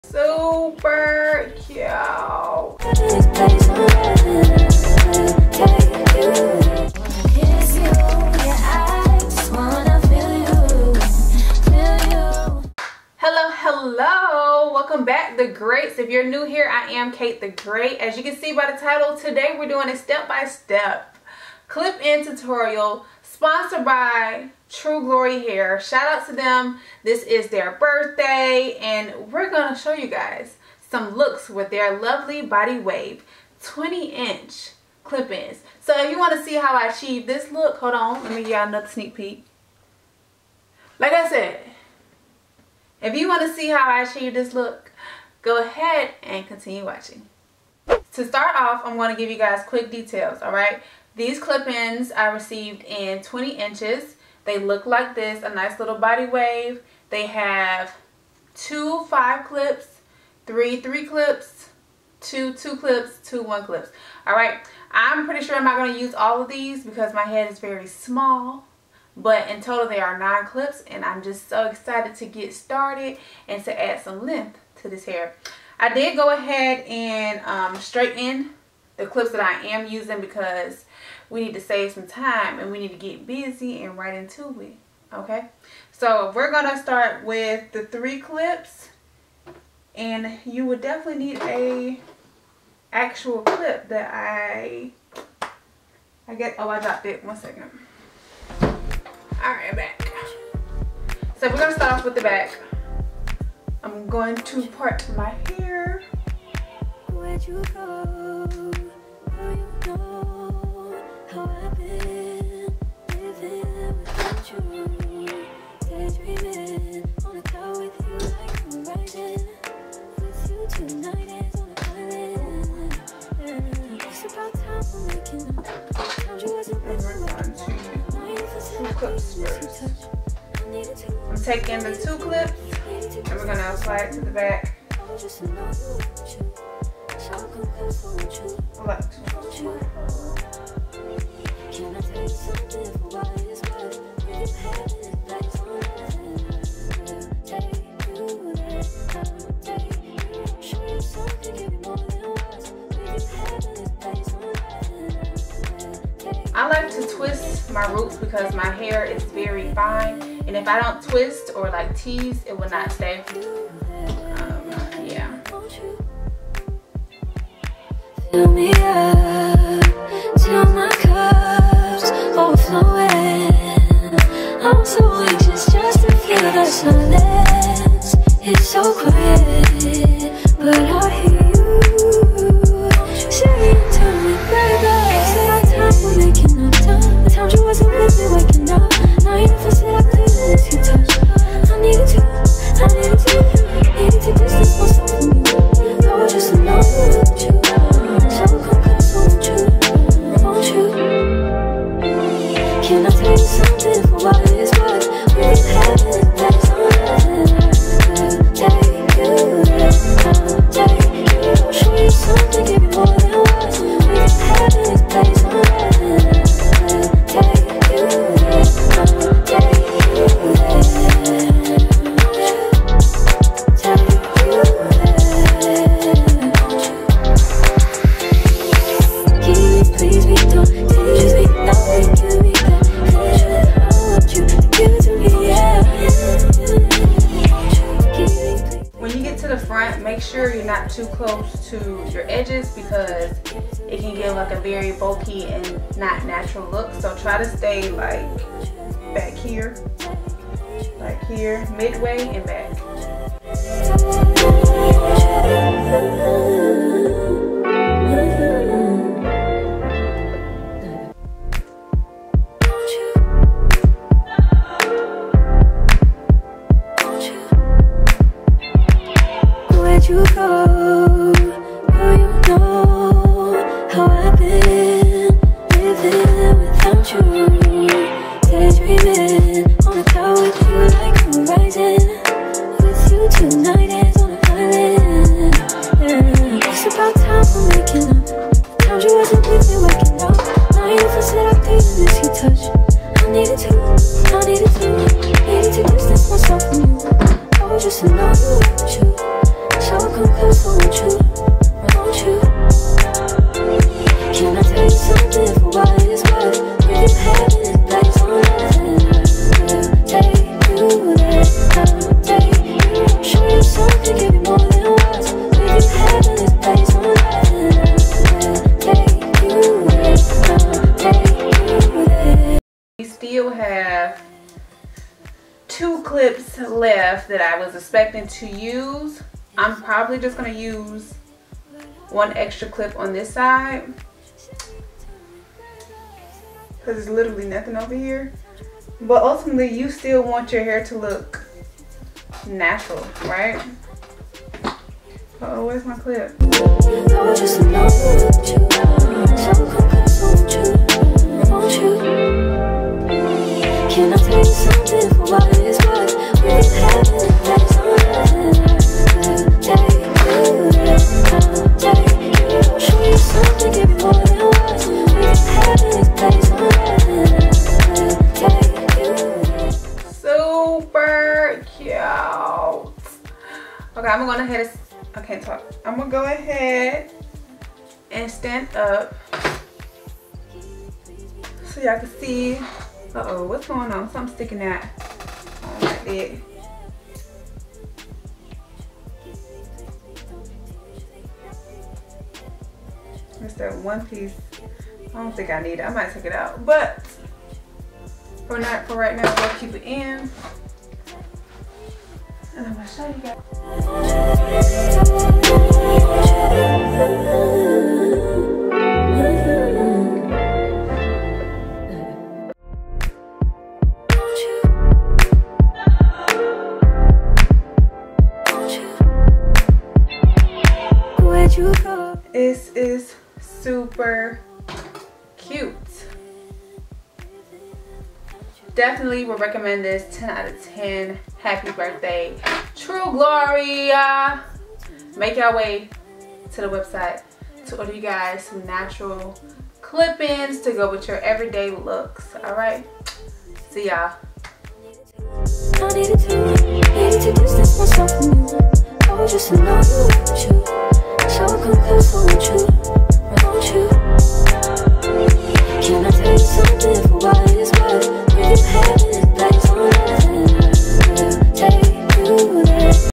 DETAILED CLIP IN HAIR TUTORIAL | TRUE GLORY CLIP-INS
Hey the Greats! We are back with a hair tutorial. I am going to show you how to get longer hair in minutes with True Glory Clip-Ins. Let me know if y'all have any questions below. Enjoy!!

True Glory is celebrating their 12 birthday all month long!

Hair: 20" Body Wave Clip-Ins
Let’s Be Friends!